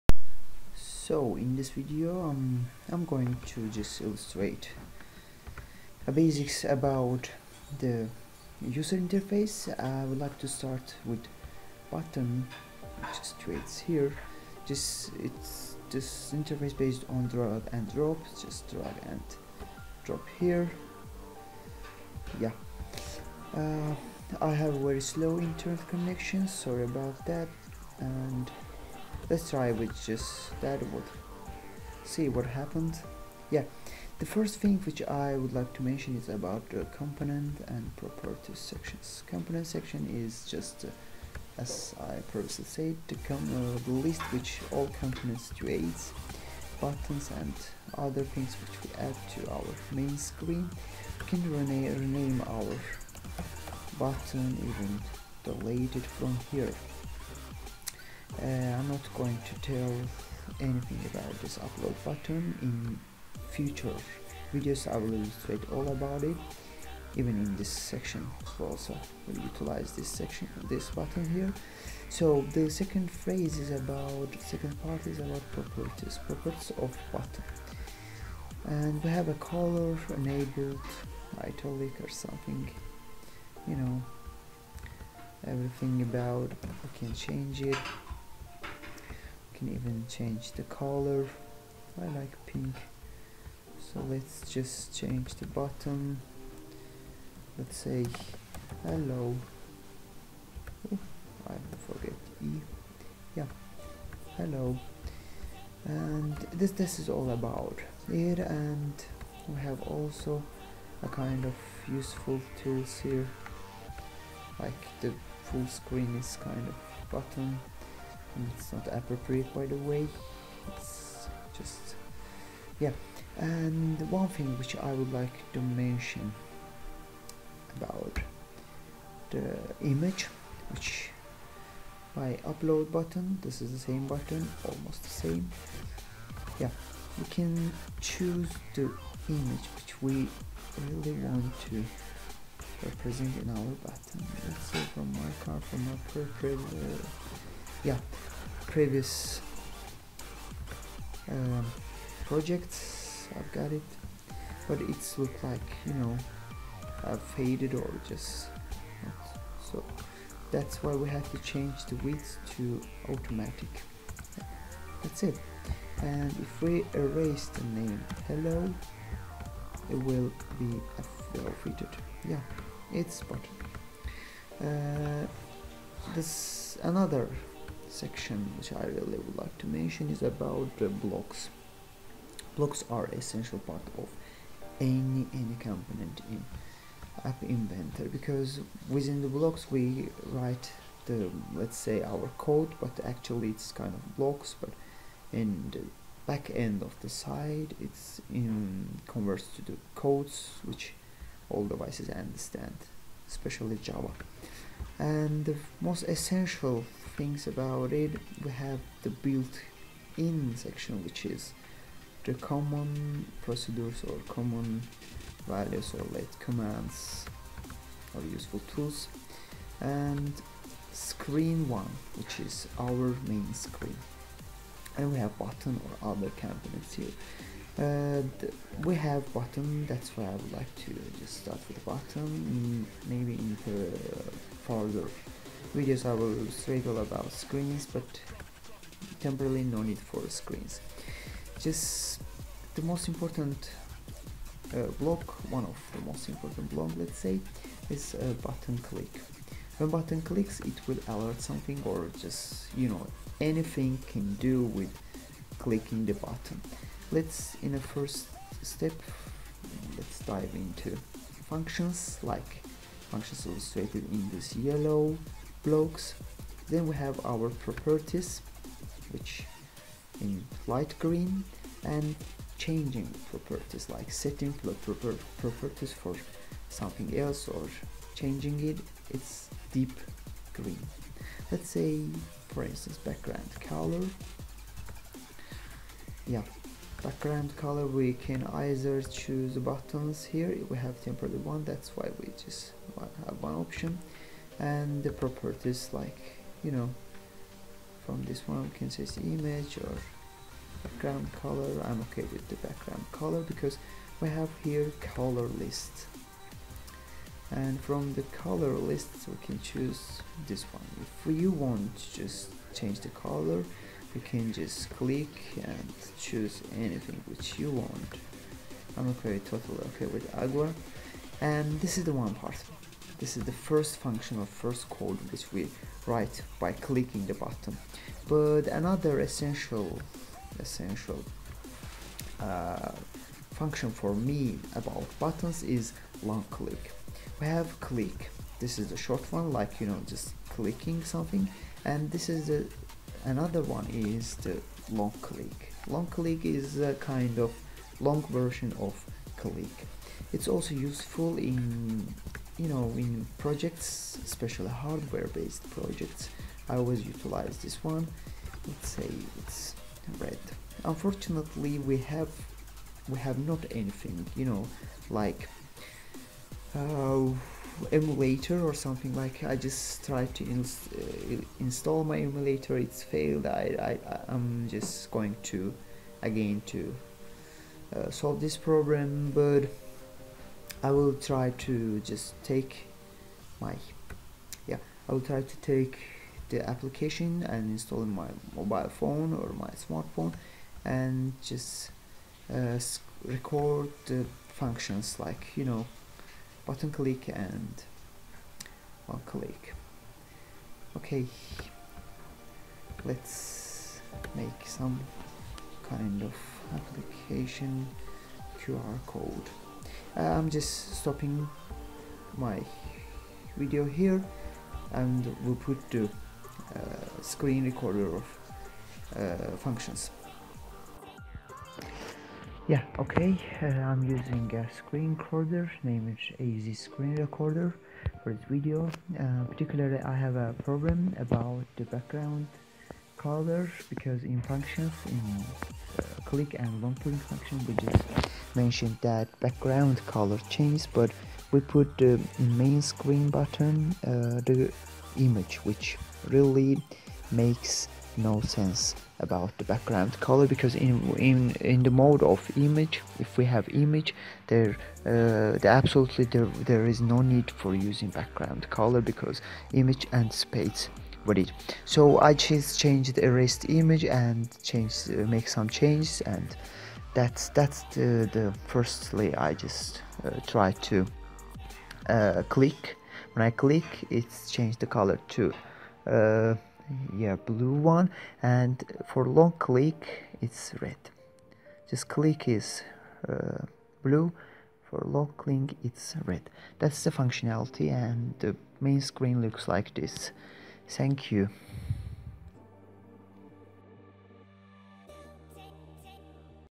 MIT App Inventor Step by Step #2 - Introduction to User Interface (Buttons)
MIT App Inventor Step by Step #2
Introduction to user interface, buttons, button properties, functions and ect. Then making function blocks and running it on Smartphone.